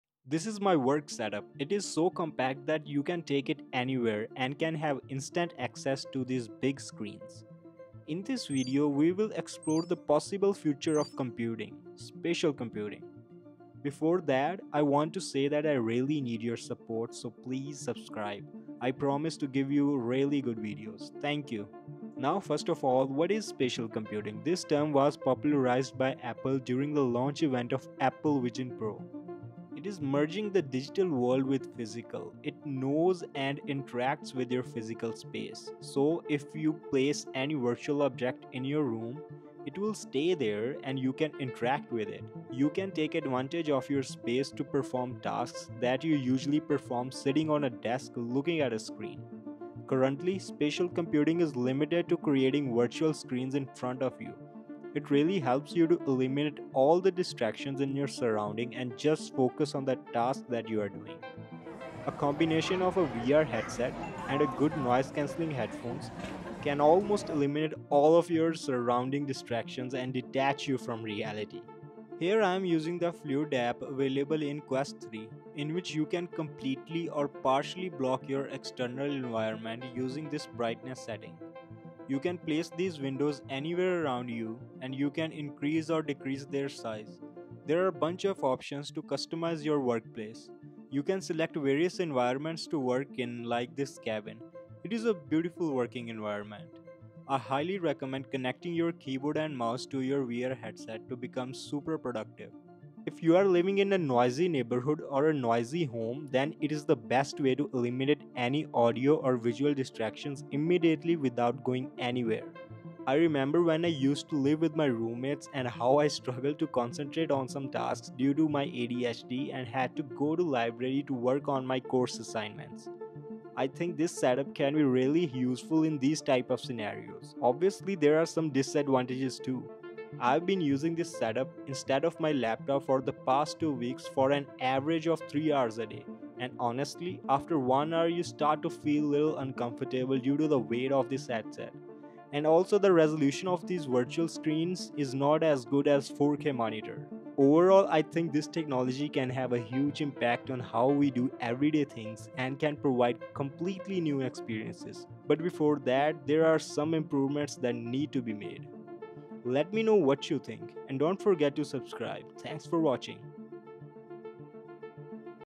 Spatial computing on Quest 3? Like VisionOS on Apple Vision Pro
I am trying to explore if Meta Quest 3 can do spatial computing like Apple vision Pro and if it is just a gimmick or you can actually use it in your daily life.